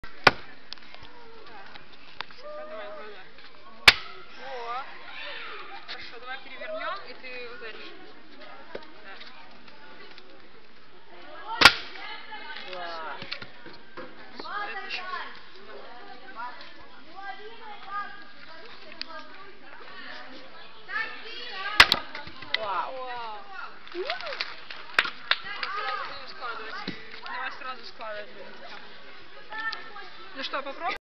Девочка Рубит Дрова
We were at camp in Belarus. Me and my sister wanted to chop wood. They didn't want to let us because we were girls, and in Belarus, there is a steriotype girls are week. After we told them it was discrimination, they finally let us. This is a video I took of my little sister chopping wood